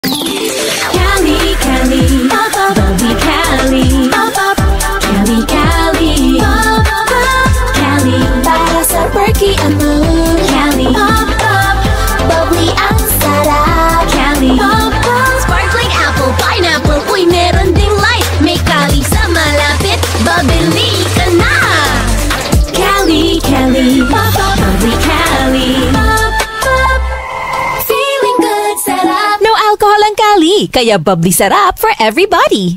Cali Sparkling Water Radio Commercial (2023) (REVISED)
This is the more new jingle of Cali

No Alcohol ang Cali, kaya bubbly sarap for everybody!

ThouShallNotSteal (Exodus 20:15)
HuwagMagnakaw (Exodus 20:15)